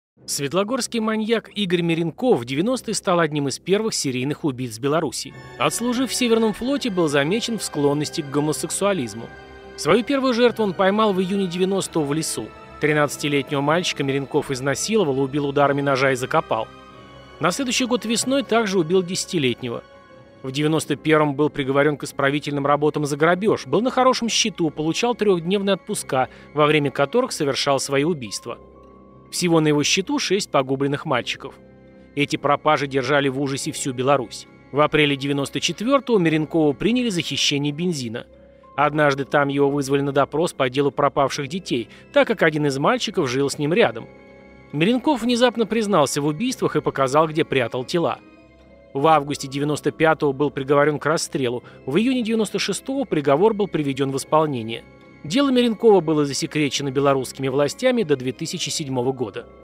Игорь Миренков, запугавший всю Беларусь. Рассекреченное дело. Вся история за 60 секунд
Игорь Миренков, запугавший всю Беларусь. Вся история за 60 секунд